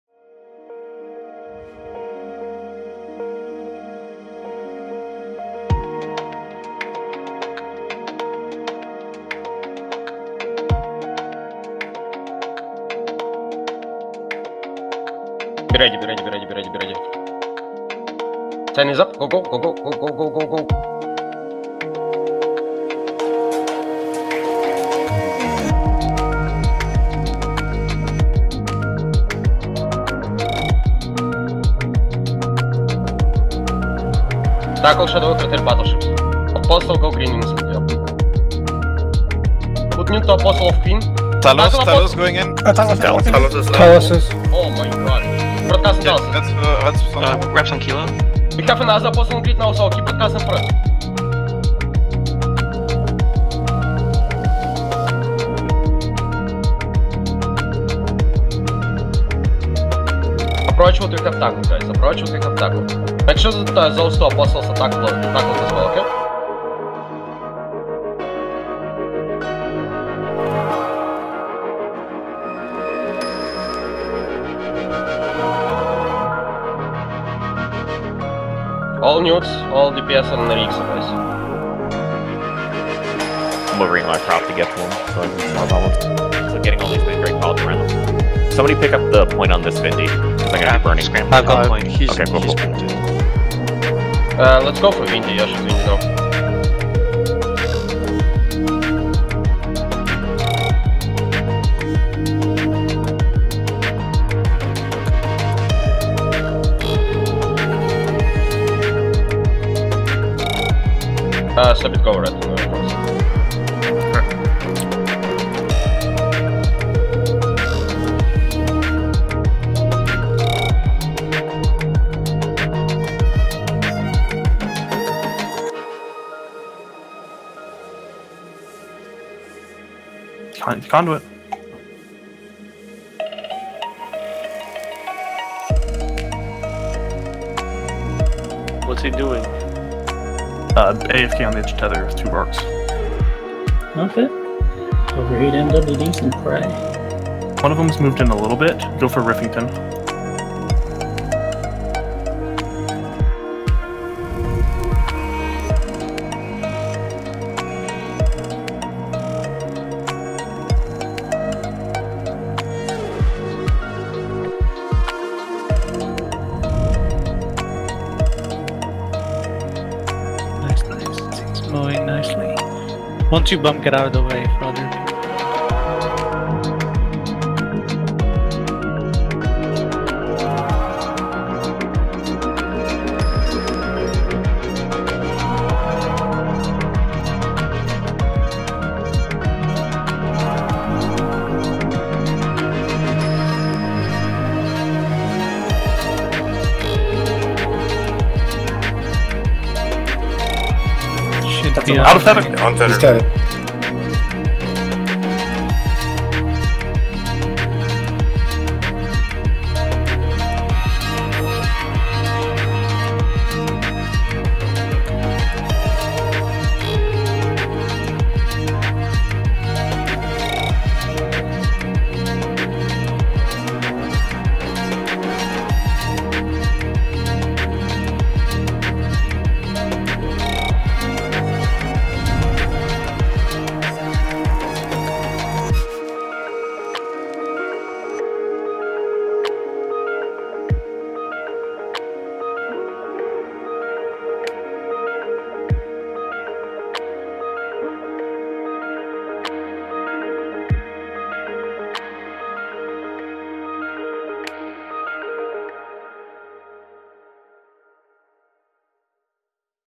Snuffed out - BLOPS Vol 3. Fleet fight/Rorqual kill
A couple blacks Ops battleship hotdrops. First one is a fleet fight in low sec vs Shadow Cartel battleships and Talos Second is bumping two Rorquals off tether - killed the first one while the other just managed to get out. . Special thanks to White Sky and ur dunked.
Ship flown is a redeemer.